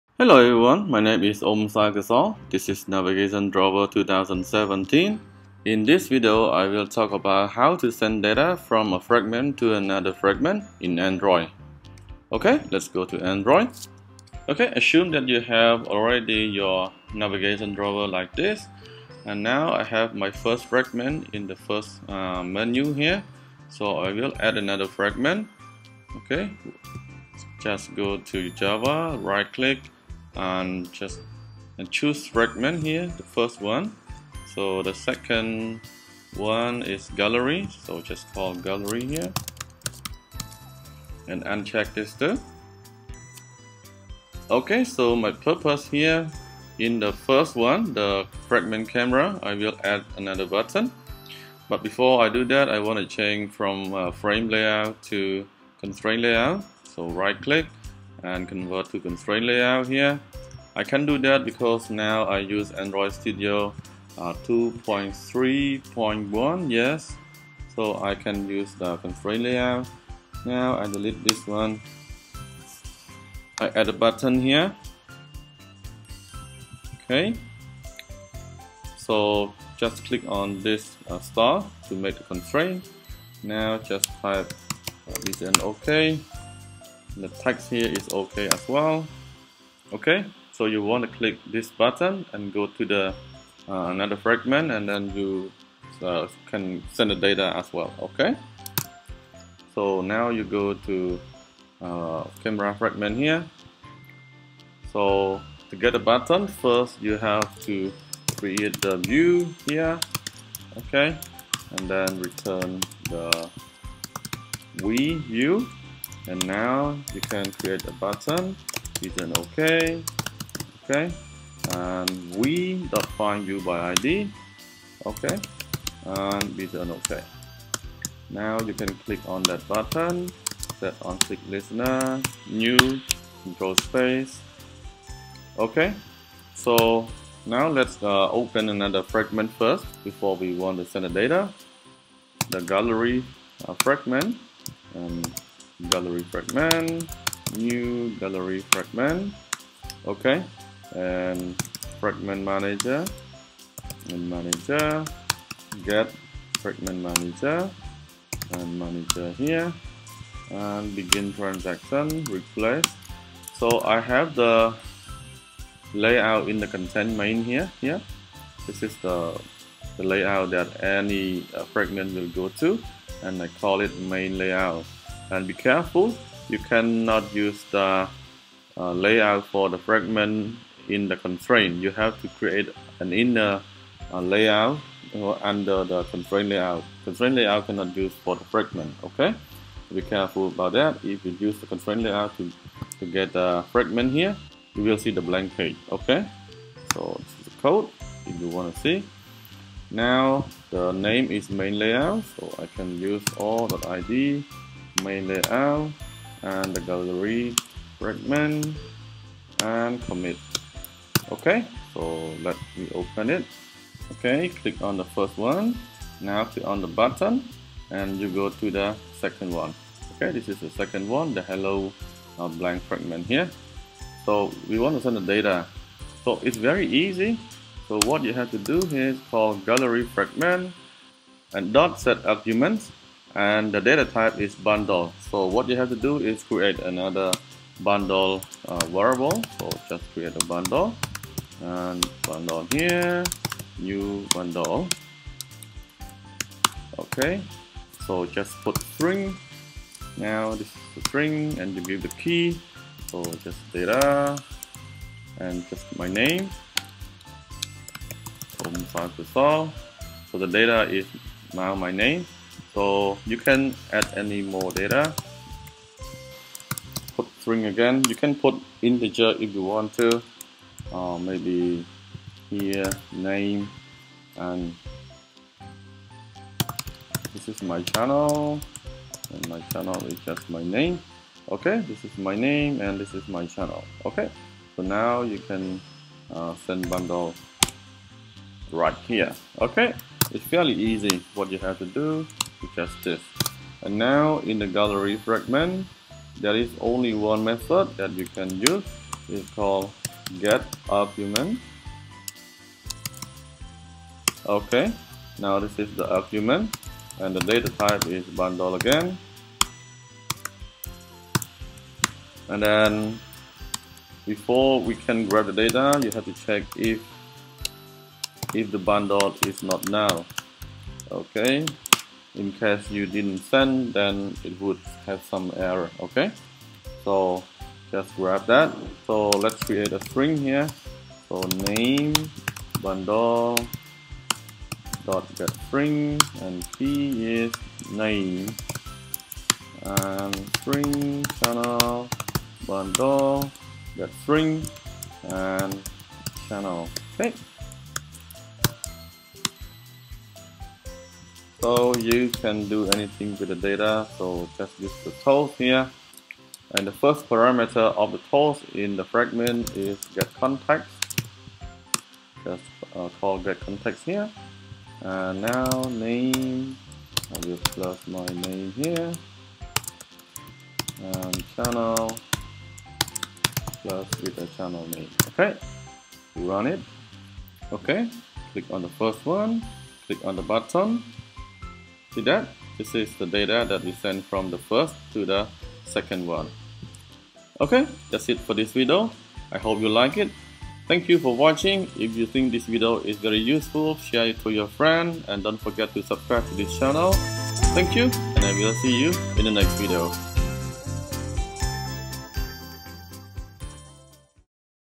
How to Send Data from a Fragment to Another in Android - Navigation Drawer
You will learn how to send data from one Fragment to Another Fragment in Android. This tutorial is for beginners. Usually you use Intent to send data from one Activity to another Activity but here it is a little different, from a Fragment to another one. Actually, what you need is just a method setArguements() with a Bundle parameter. You create an instance from Bundle and you send data through it. And in a received Fragment, you can get the data from getArguments() method with the return type of Bundle. It is fairly easy. If you know in detail, please watch the video tutorial.

If you want to see cool videos like this about Android Studio, please follow me at:
For English version:

Oum Saokosal